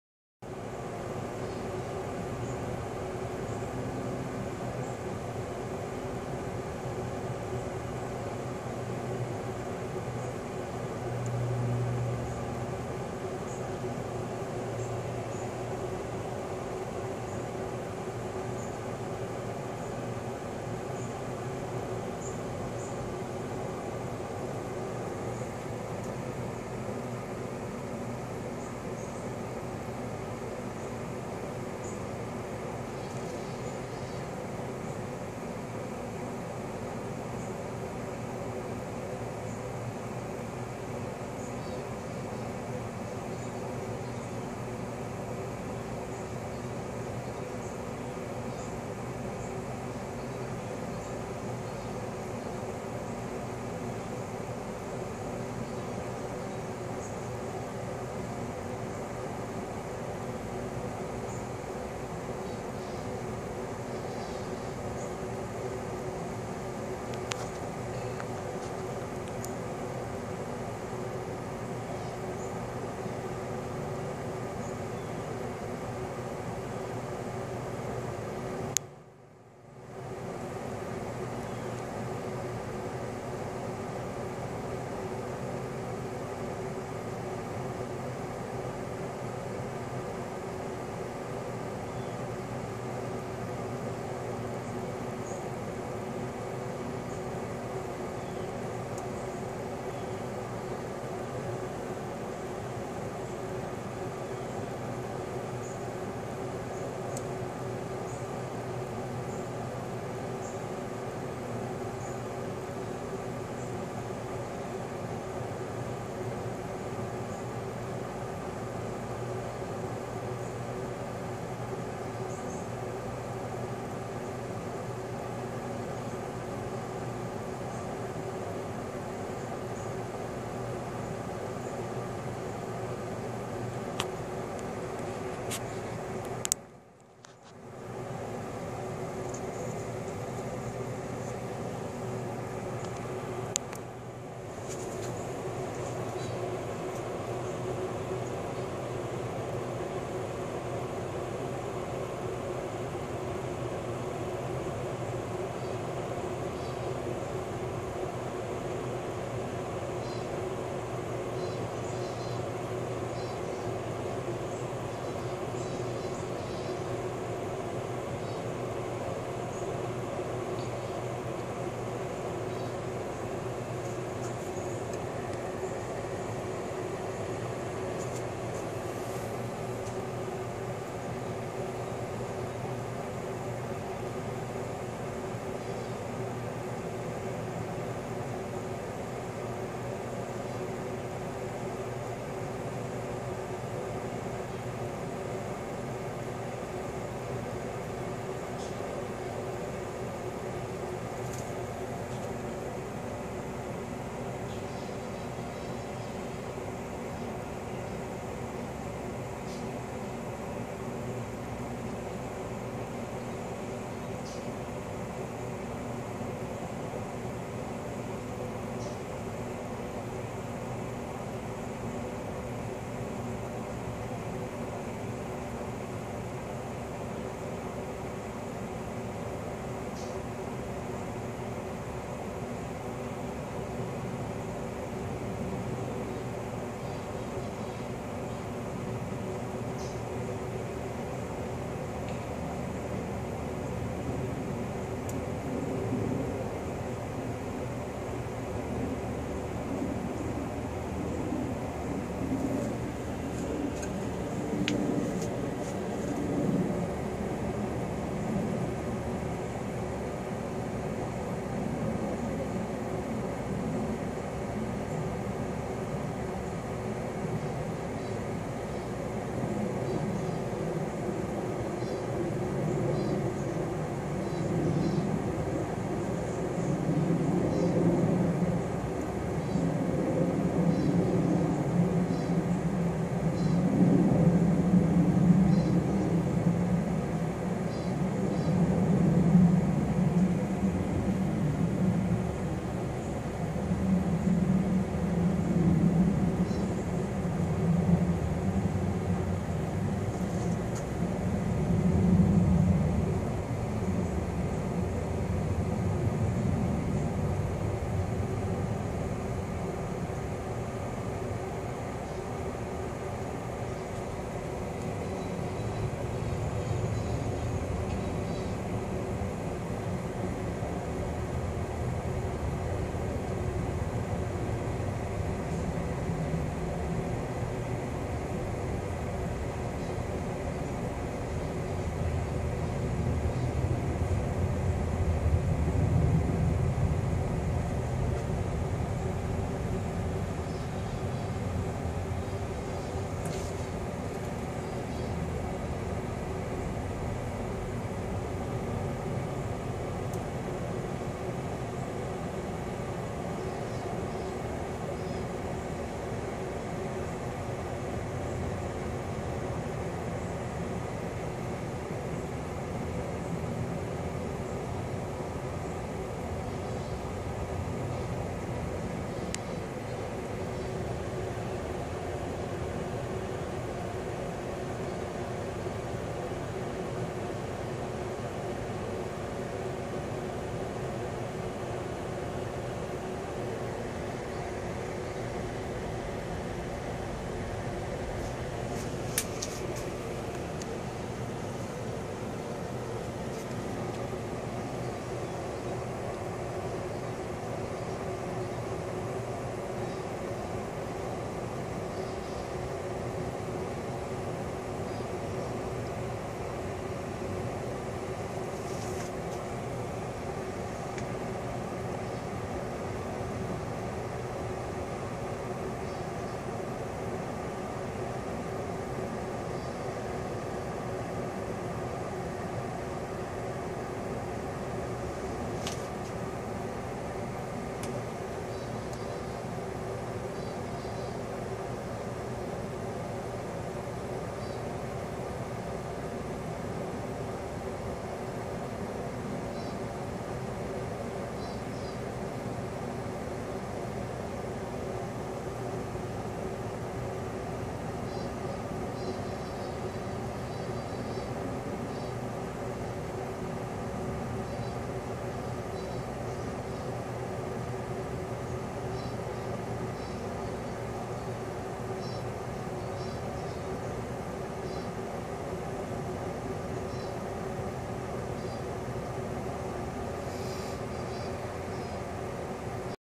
Zebra longwing caterpillars in different stages
These guys were layed on my new passion vine right after I bought it by the longwings that have been on my property for some time because of the wild Lantana and Shepards needle.